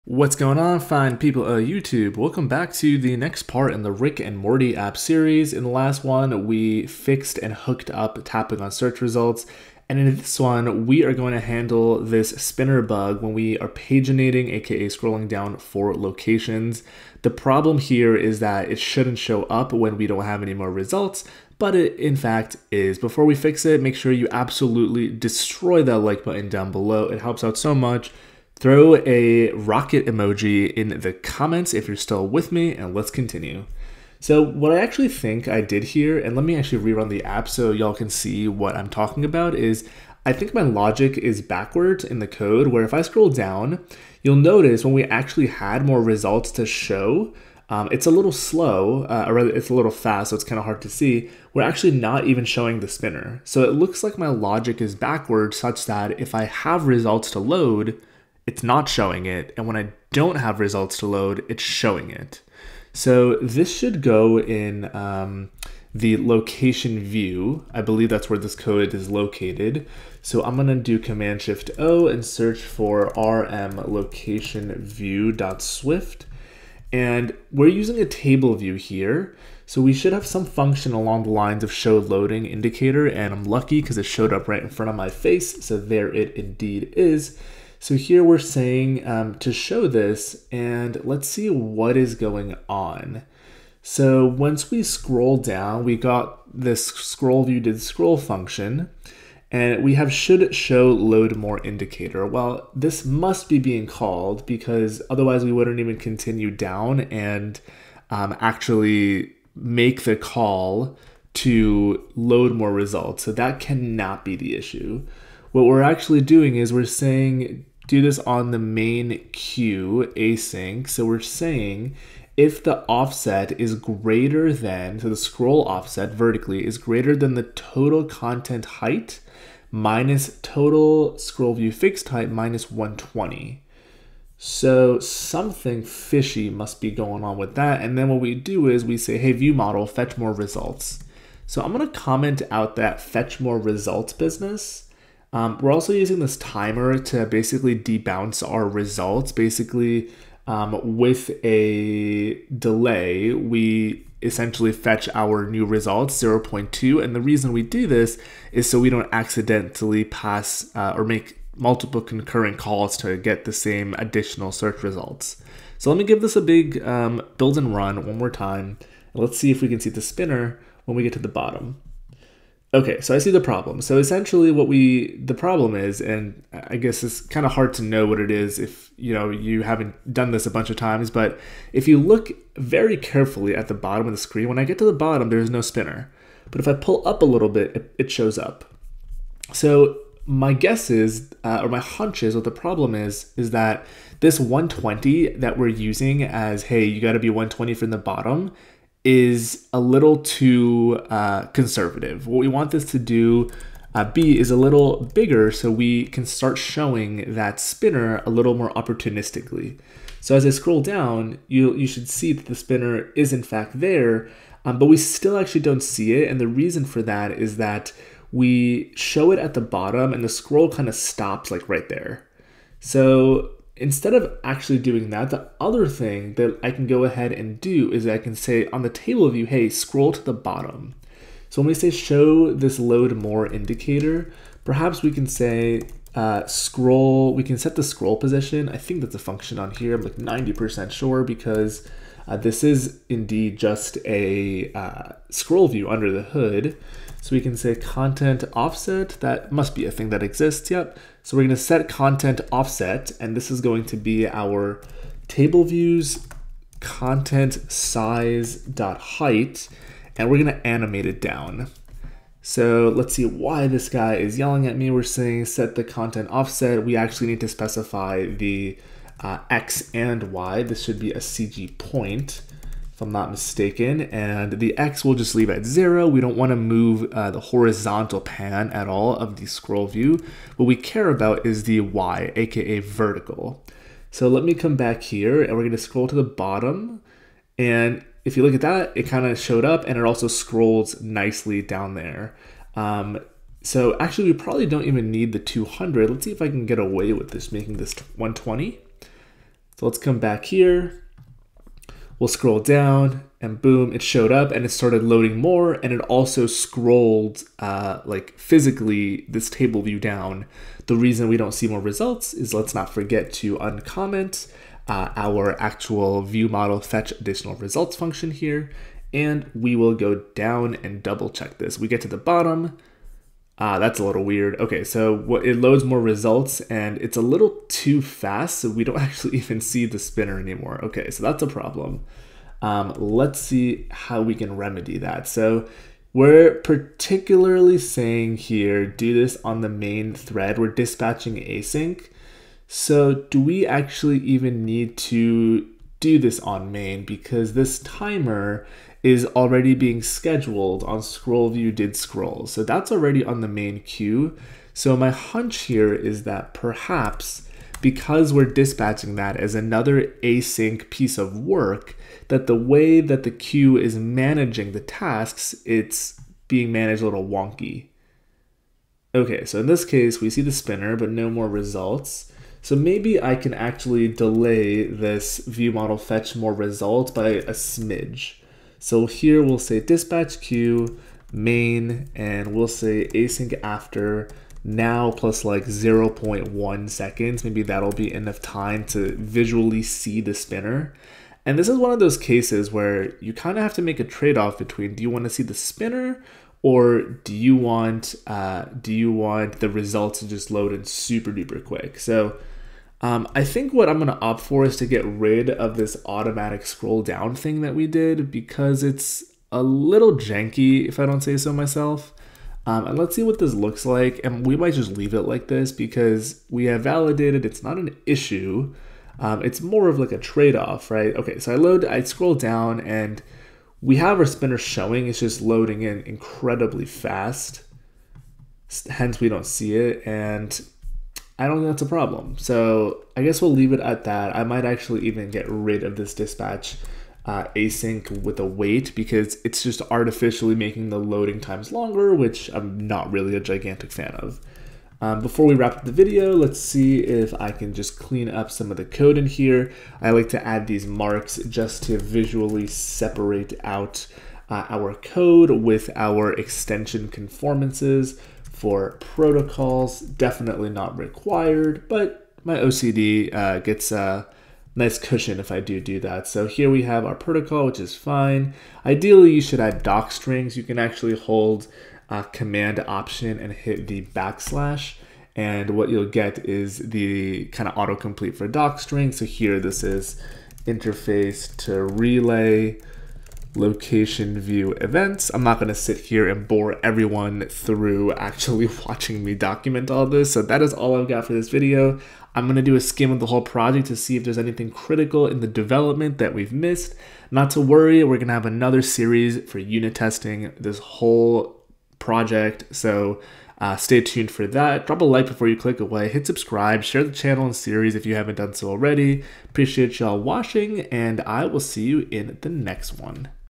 Build FULL iOS App in Swift: Part 58 (Rick & Morty | 2023) – Fixing Bugs
In this video we will continue building the Rick and Morty app. We'll tackle fixing the loading indicator for Location pagination UI.